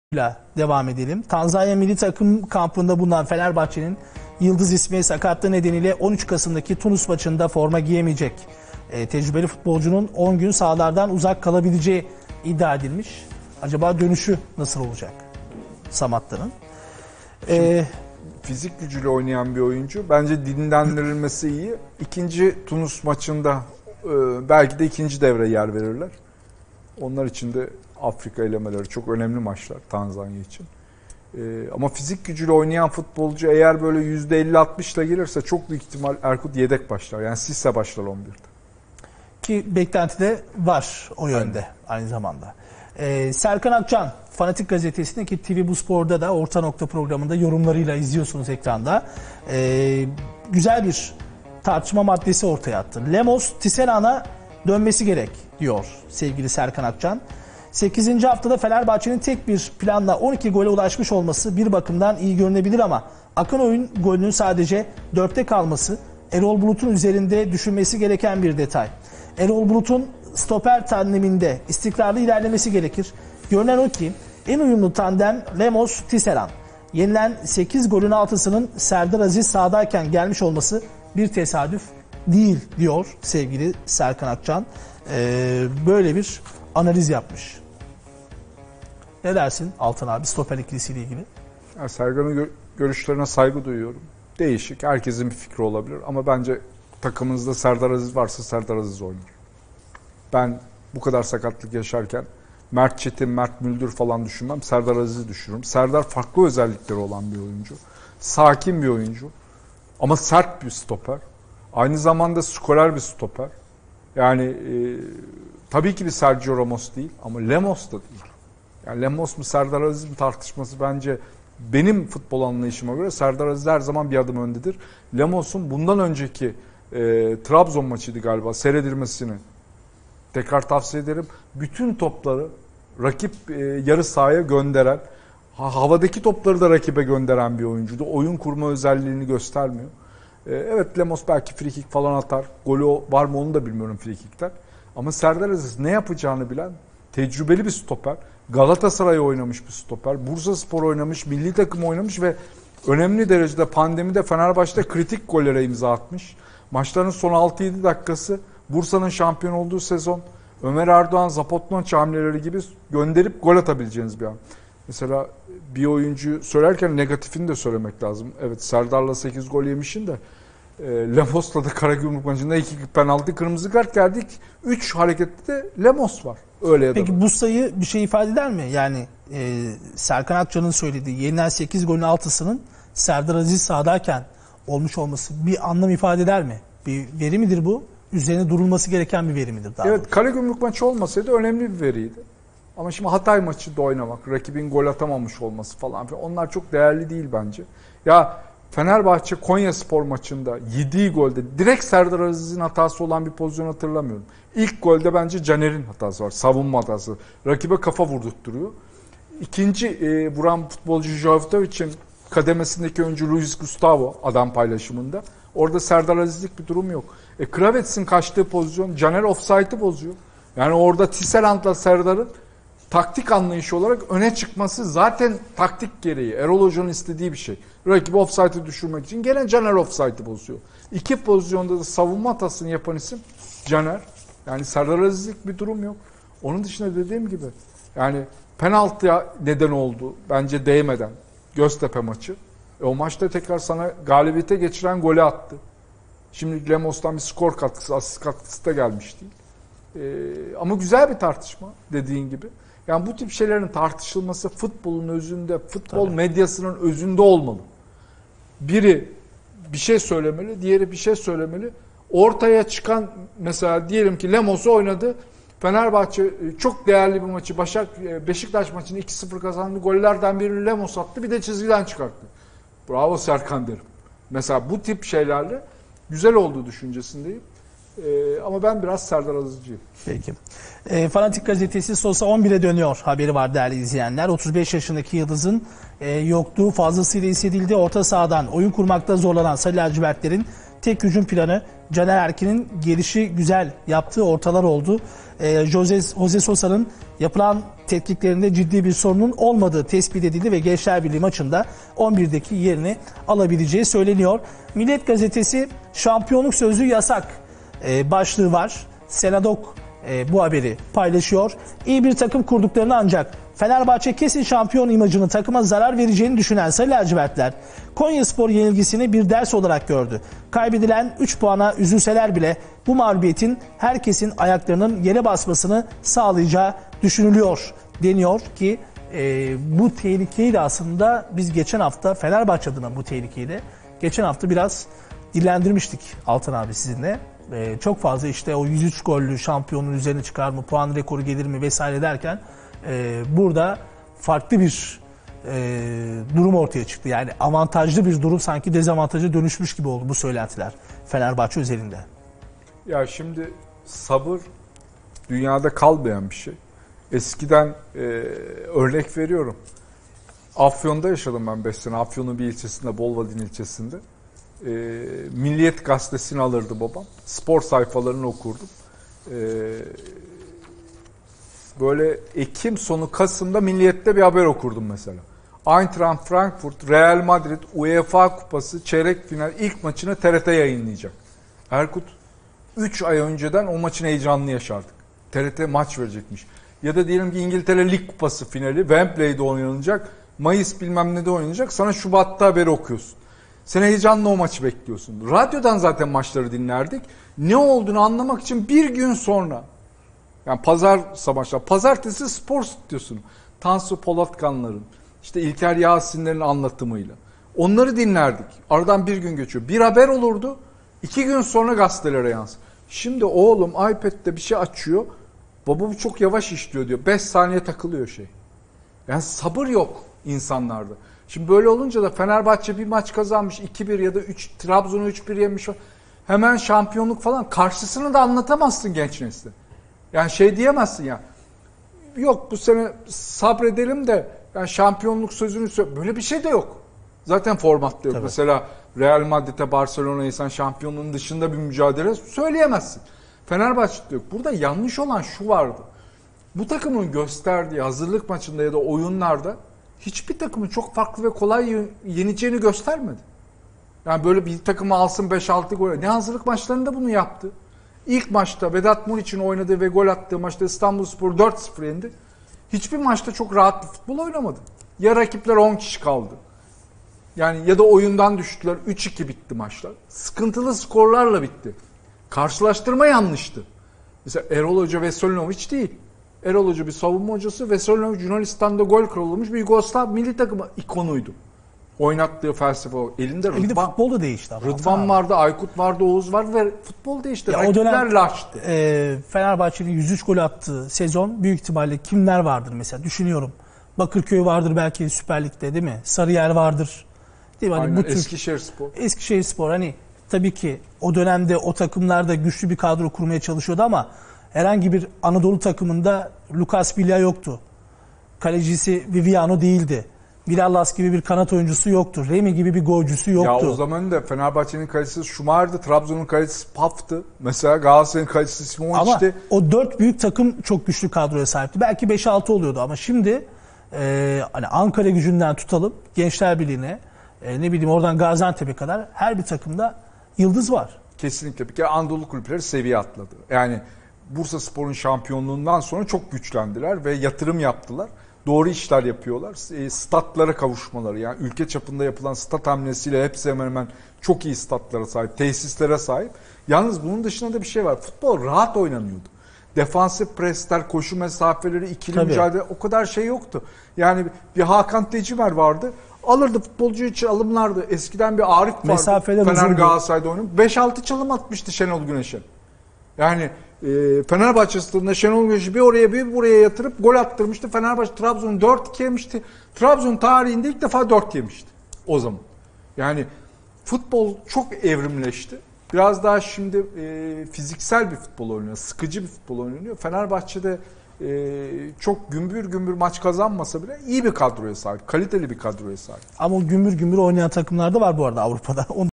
Fenerbahçe Gündemini Altan Tanrıkulu Değerlendirdi | Spor Gazetesi - 11 Kasım 2020
Spor Gazetesi'nde Fenerbahçe gündemini Altan Tanrıkulu ile Erkut Öztürk değerlendirdi.